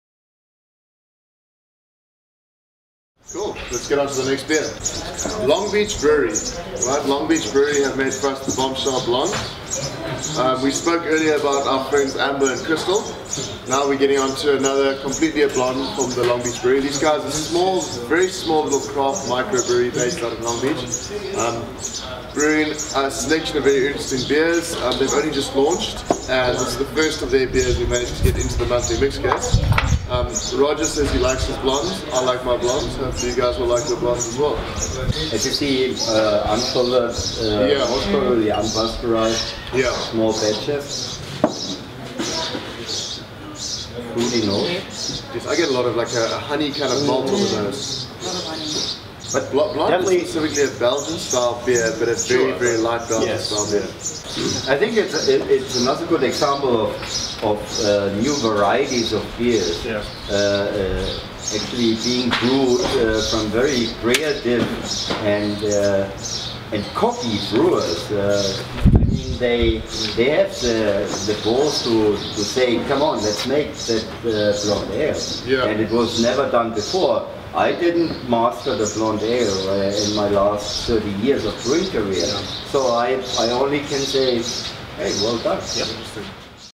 LoB Nov. Tasting Ep 4 Long Beach Brewery- Bombshell Blonde
The Bombshell Blonde is a Belgium style blonde ale, with a South African twist. The aroma displays typical warm, honey tones, but the tropical notes of local hops, Southern Passion, add a passion fruit layer. These flavours follow through on the palate, and the low bitterness makes this a great all rounder for all beer drinkers. A real session beer.